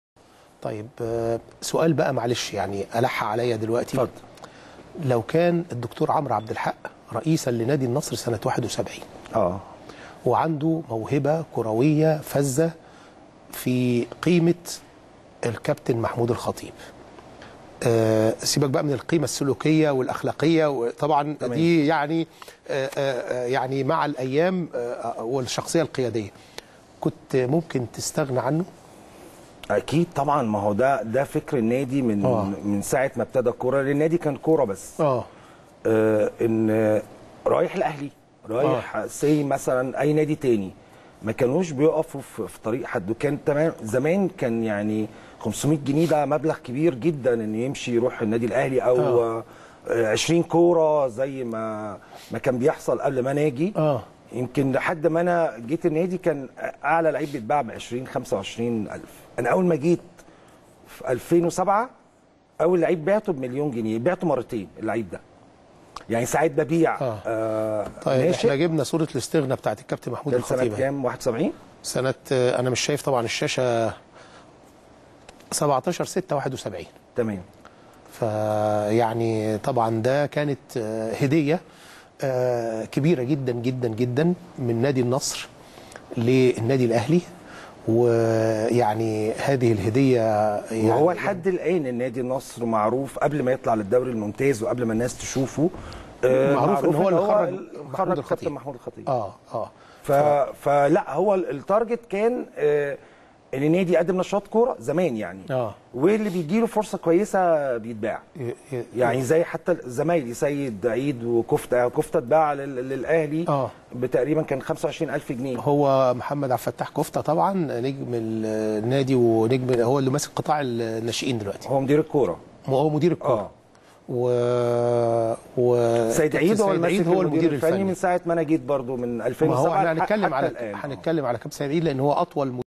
عمرو عبد الحق يجيب " ماذا لو كان رئيس لنادى النصر هل كان يوافق على بيع الخطيب لنادى الاهلى "
عمرو عبد الحق يجيب على سؤال " ماذا لو كان رئيس لنادى النصر هل كان يوافق على بيع الخطيب لنادى الاهلى "

التردد: 12341 أفقي.
القمر: نايل سات.
معامل الخطأ: 5/6.
معدل الترميز: 27500